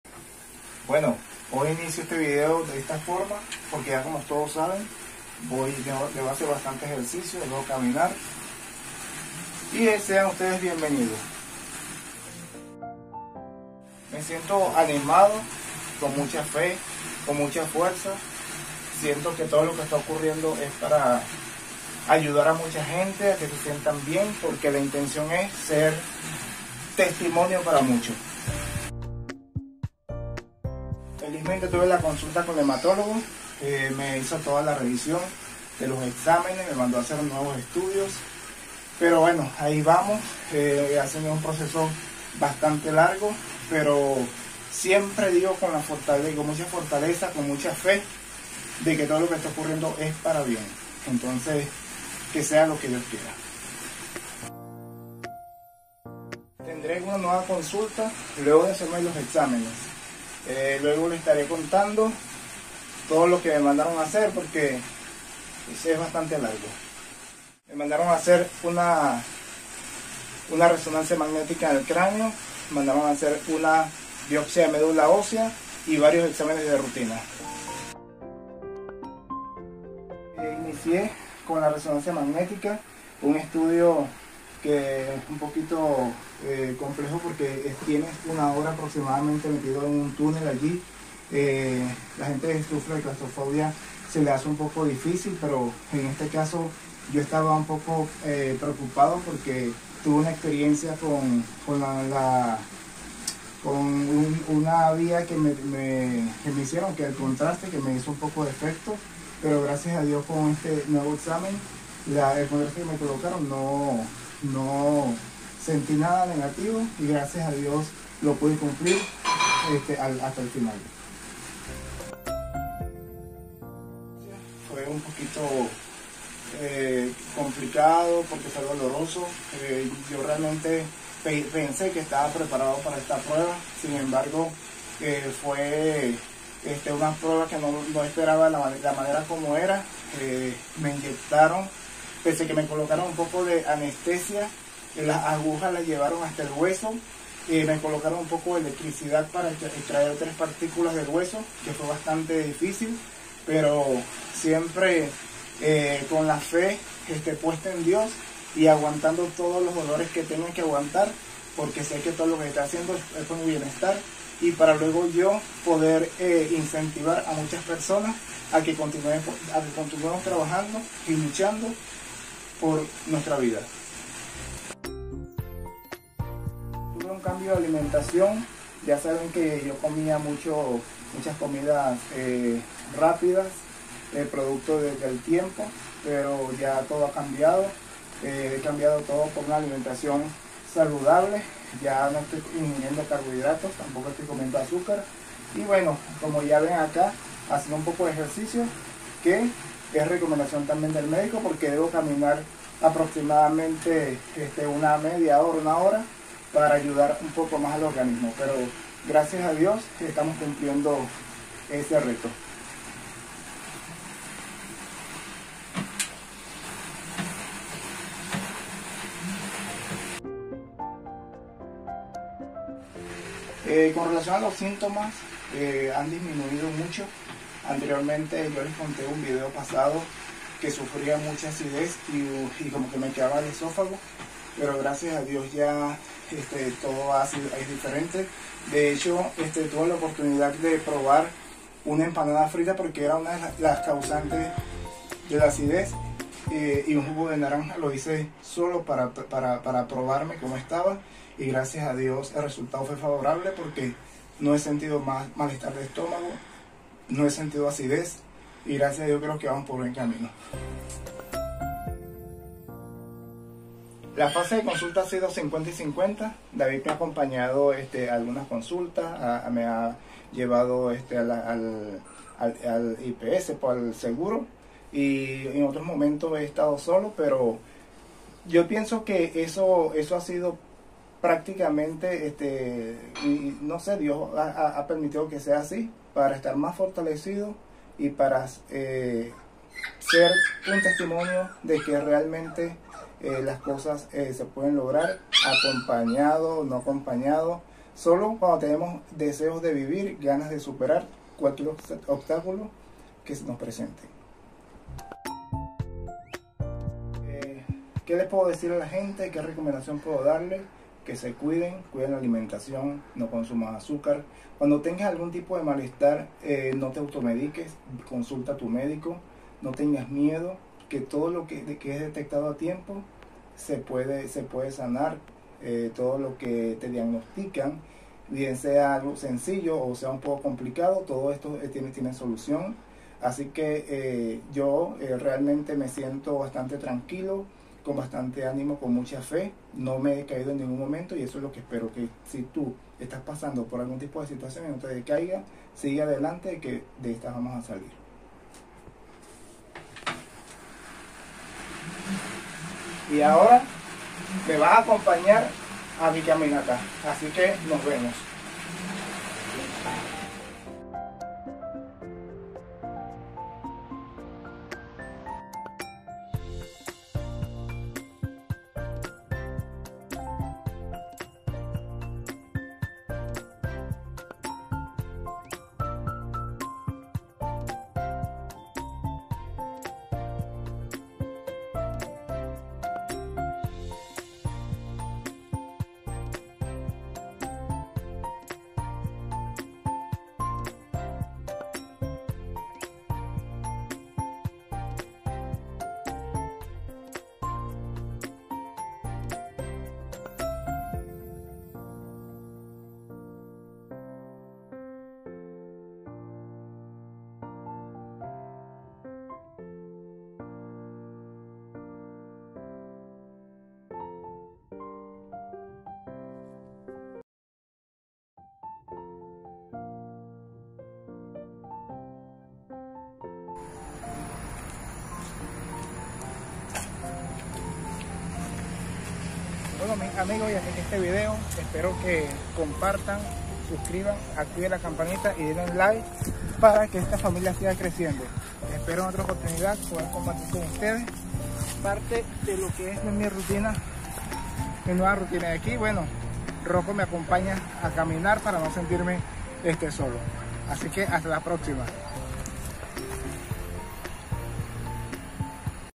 Me hicieron una biopsia de médula osea.
En este video se busca incentivar a los espectadores para que tengan una vida saludable y preventiva. acudan al médico y realicen los estudios de rutina.